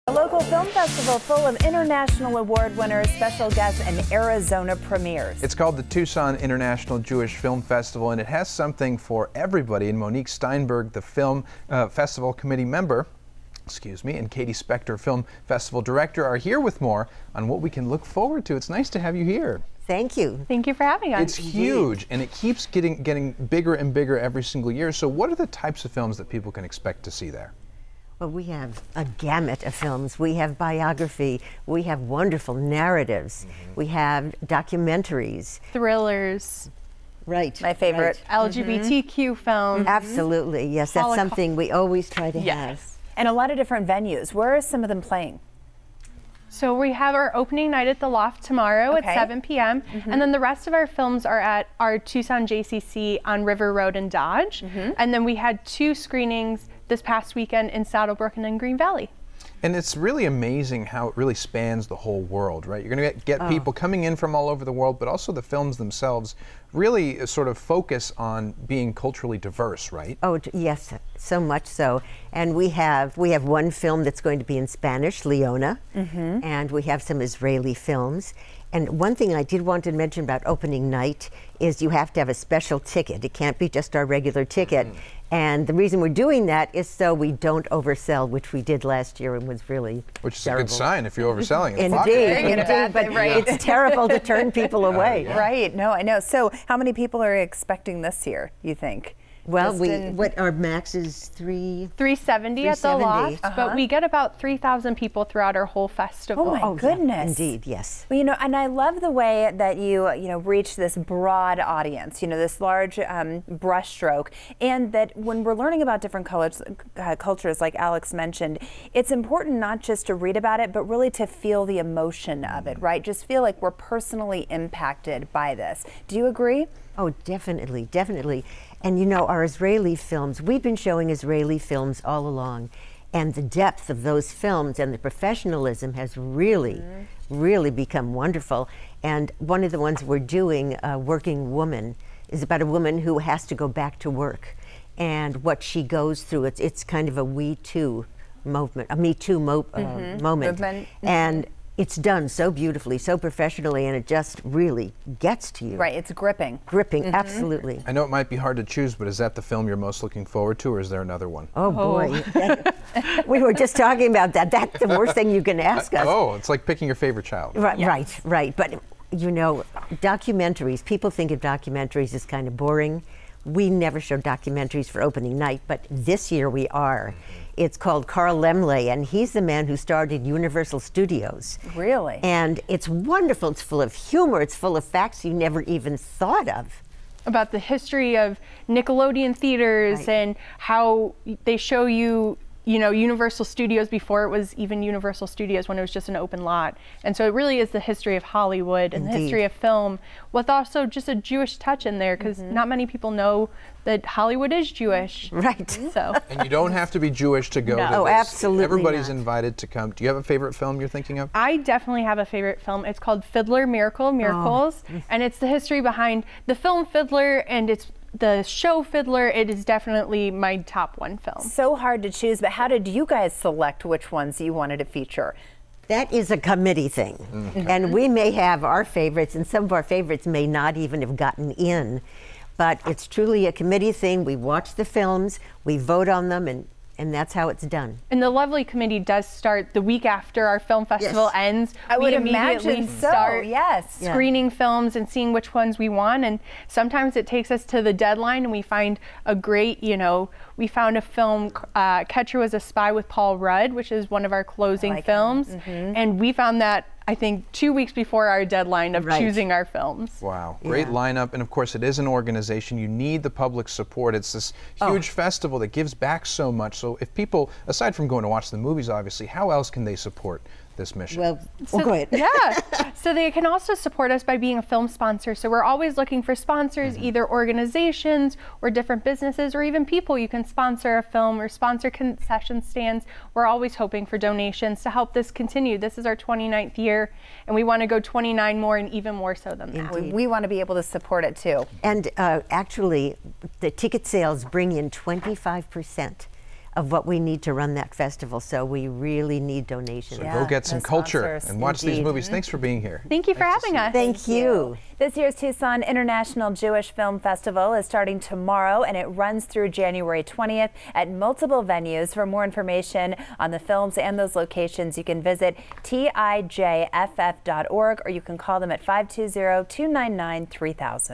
A preview of Tucson International Jewish Film Festival 2020
One of the country’s longest-running Jewish film festivals, one of Arizona’s longest running film fests, and one of Tucson’s most popular cultural arts events